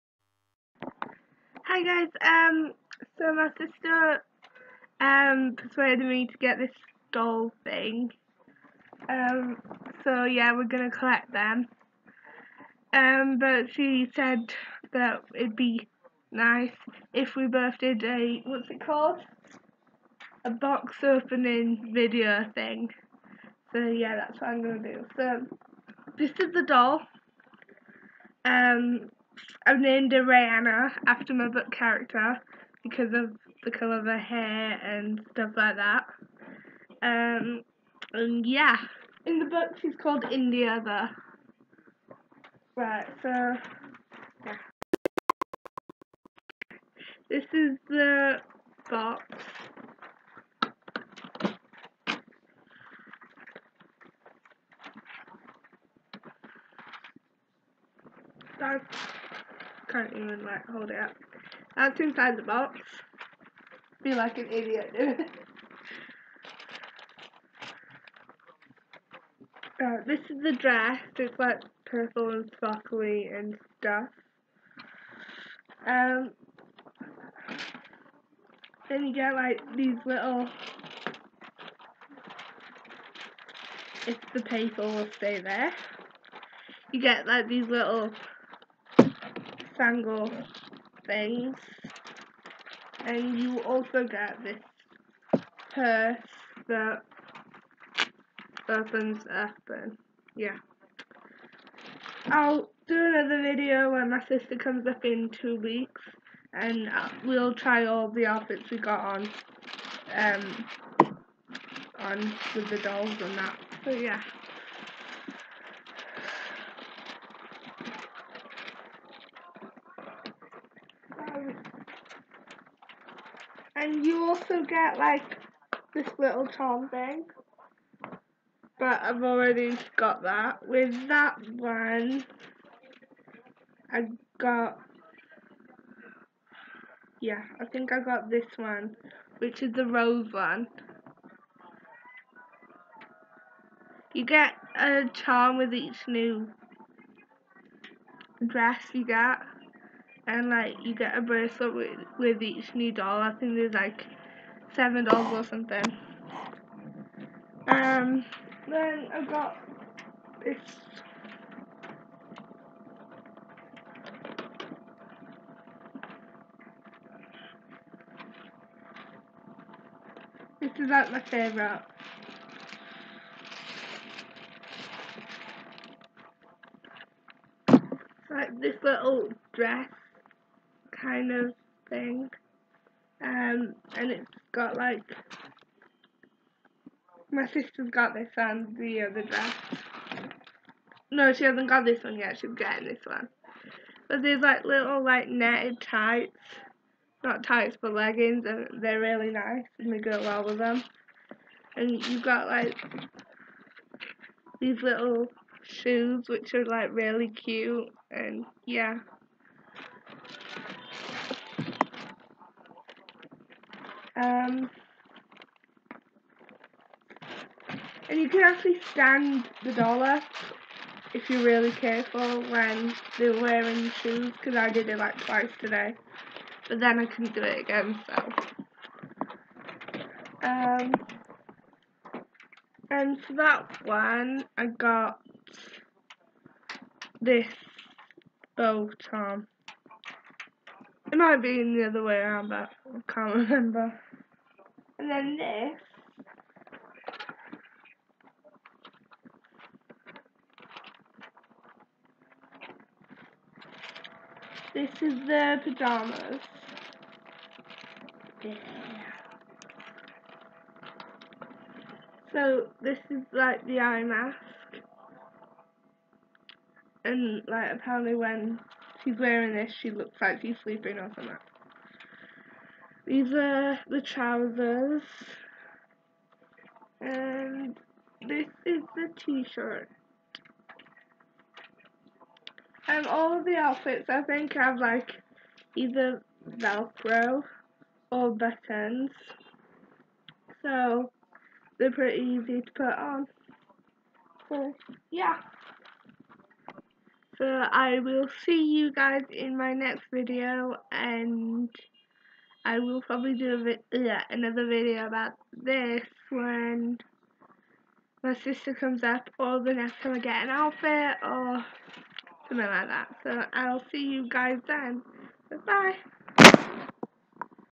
Box opening of 'design a friend'
Hey!
So me and my sister needed to 'bond' over something, so this is it!
Please, no hate. I hardly talk to my sister so this is really good for us.
P.S- Sorry for my hair!
Eleanor xoxo